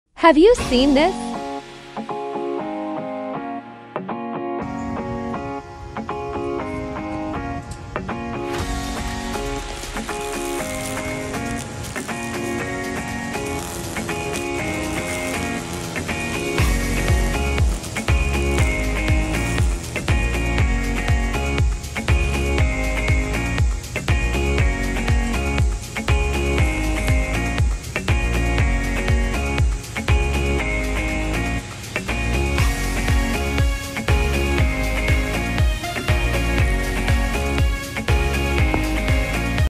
Cicada Killer
Have you seen this wasp before? Its a cicadakiller! Our tech Gina has been taking some great photos/videos recently. Check this out!